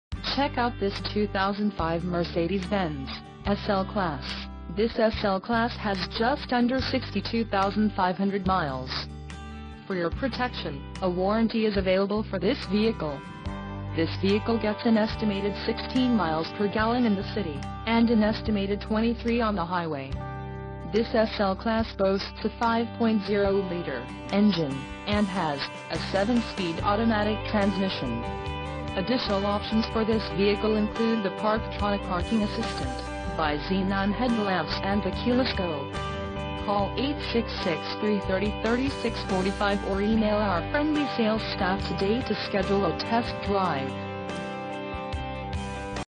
2005 Mercedes-Benz SL-Class SL500 2dr Roadster 5.0L Convertible - Rolling Meadows, IL
Exotic Motors, Rolling Meadows, IL - 2005 MERCEDES SL500 CONV'T!! NAV KEYLESS.GO A/C&HEATED-STS PDC XENONS 18"WHLS!!